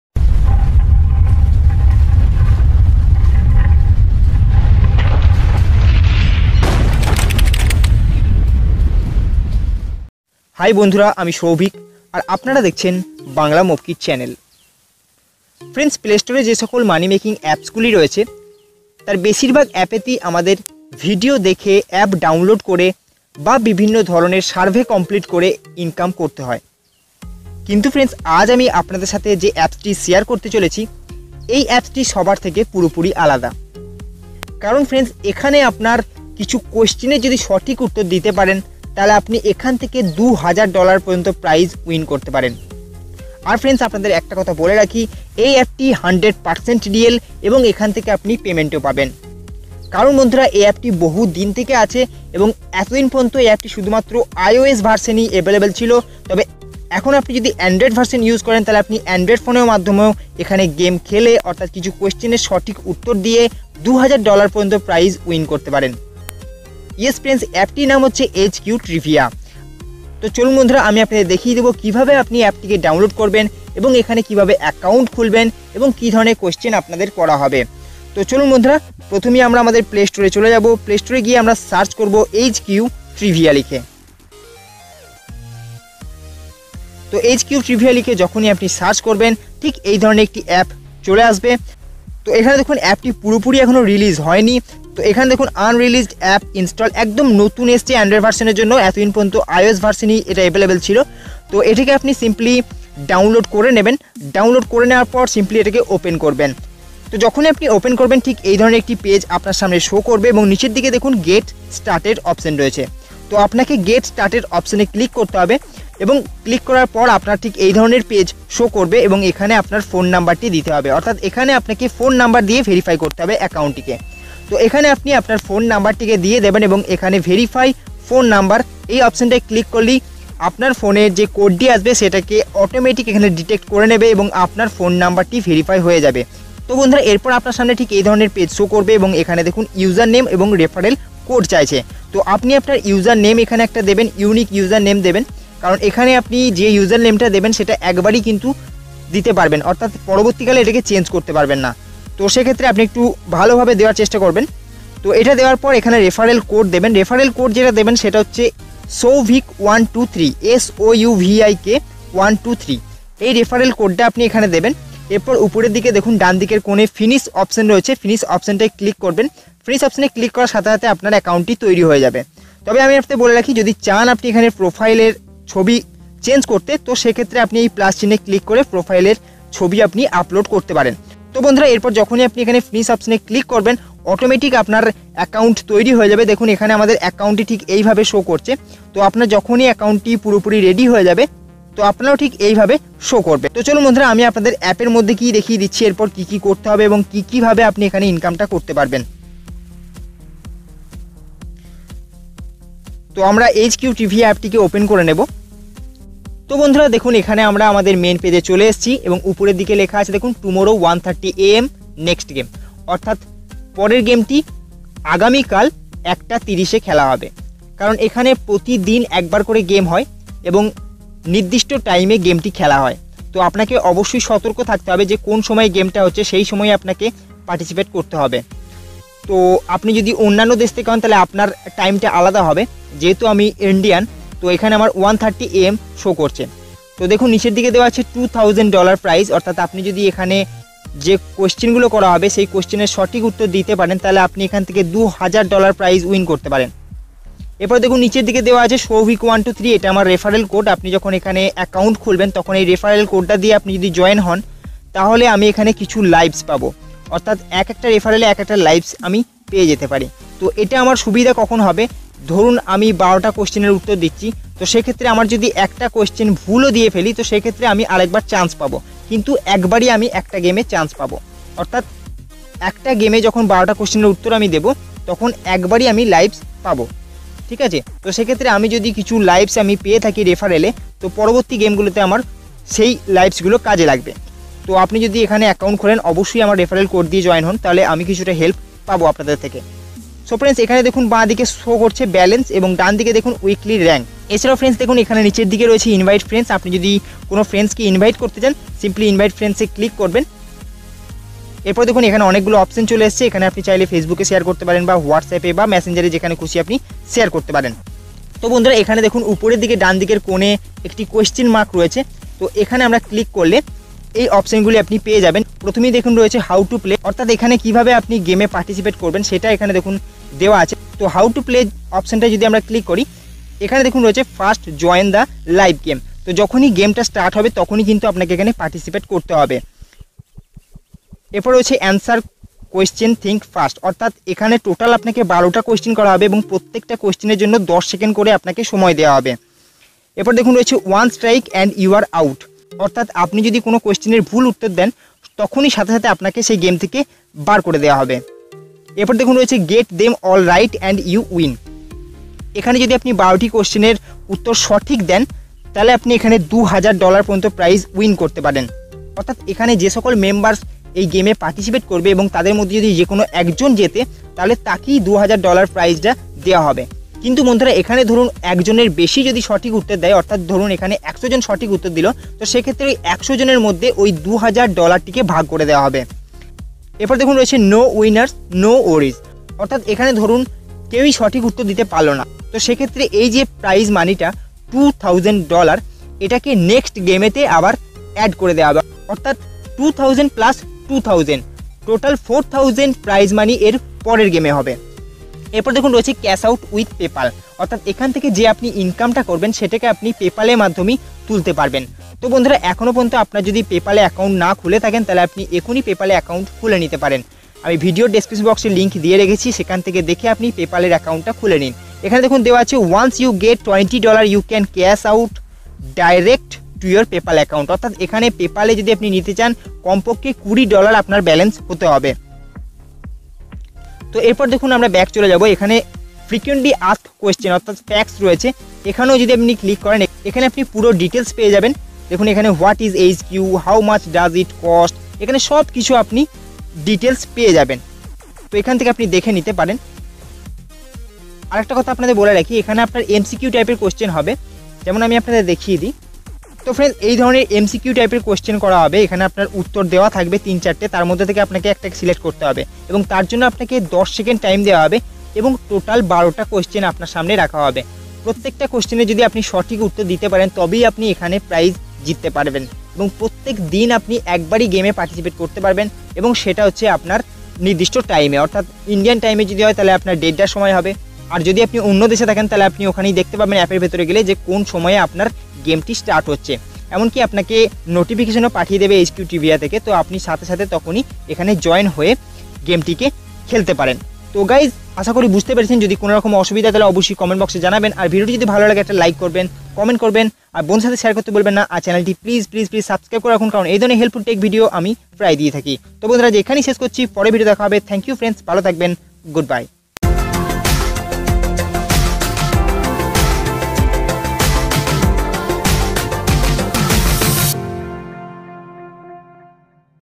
(HQ Trivia app)প্রতিদিন Game খেলে আপনিও জিতে নিতে পারেন $2000 মূল্যের prize।New app in playstore
How to earn money with quiz
How to make money on android
Hi friends,aj ami apnader sathe emon ekti share korbo,jekhane apni 12 ti proshner sothik uttor diye 2000 dollar prize pete paren.eti 1000000% real app.

Refer code   souvik123

Refer code   souvik123

Refer code   souvik123

💐💐💐   BANGLA MOBKID💐💐💐